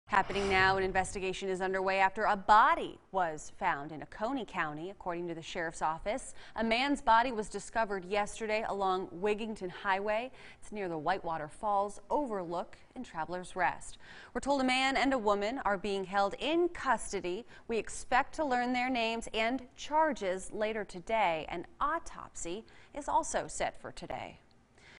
2 in custody after man’s body found along Hwy 107 in Oconee Co.
2 in custody after man’s body found along Hwy 107 in Oconee Co.